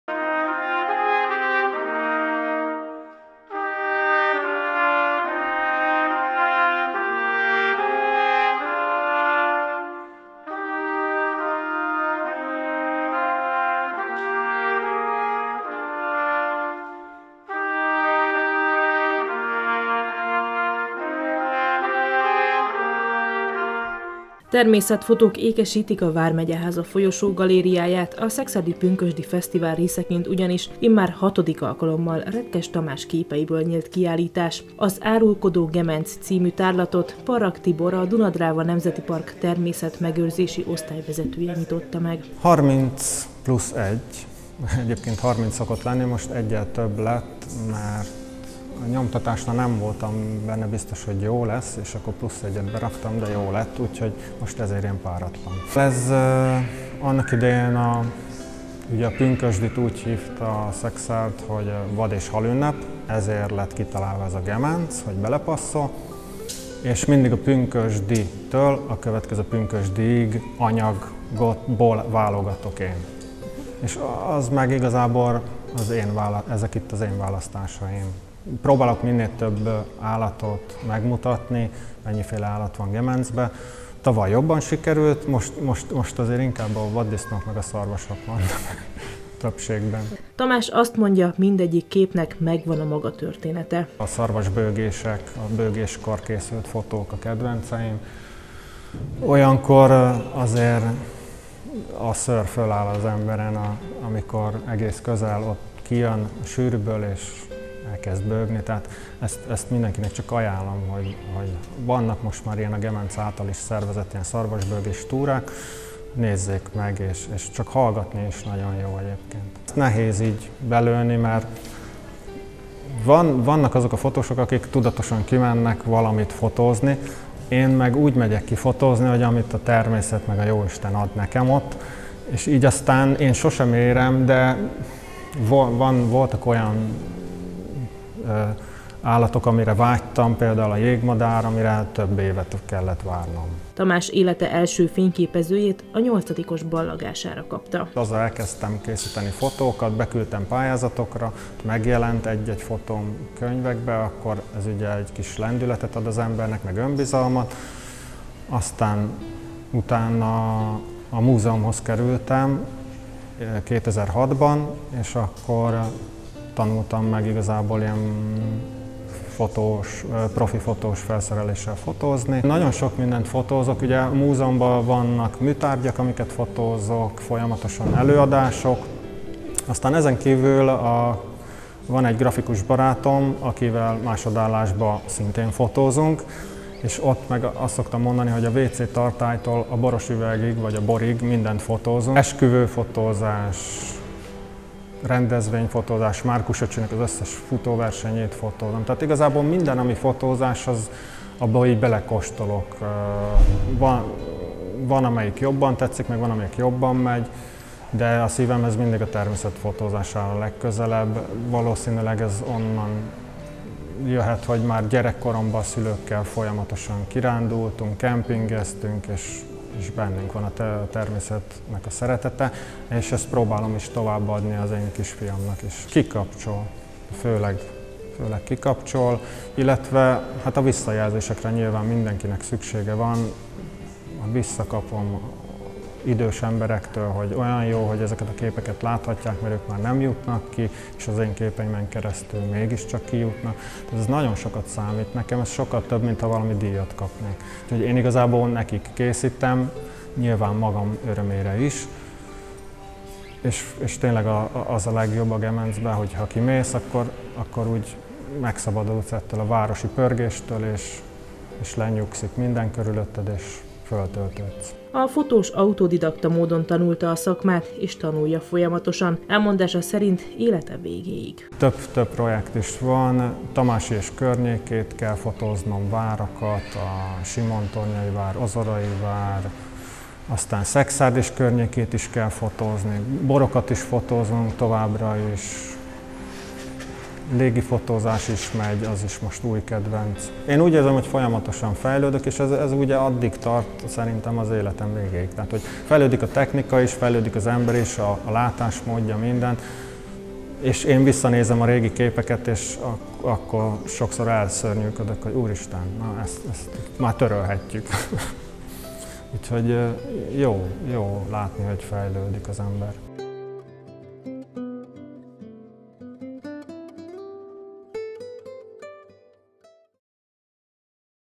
Az árulkodó Gemenc címmel nyílt kiállítás a szekszárdi Vármegyeházán
Az árulkodó Gemenc címmel nyílt kiállítás a szekszárdi Vármegyeházán. Retkes Tamás képei immár hatodik alkalommal díszítik az épület folyosógalériáját. A fotók a természetvédelmi oltalom alatt álló ártéri erdő élővilágát mutatják be.